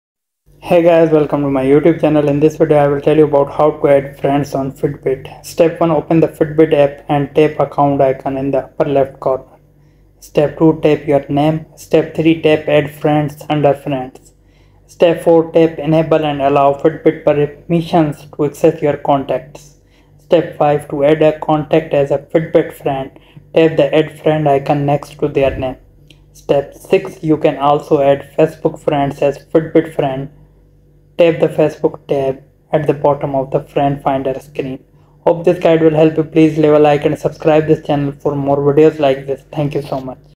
How to Add Friends on Fitbit - The Ultimate Tutorial
Unlock the full potential of your fitness journey with our comprehensive guide on how to add friends on Fitbit. Connecting with friends on Fitbit not only boosts your motivation but also enhances your overall fitness experience. In this ultimate tutorial, we’ll walk you through each step to ensure you can effortlessly add friends, compete in challenges, and share your progress. Whether you’re new to Fitbit or looking to expand your fitness circle, this guide is perfect for you.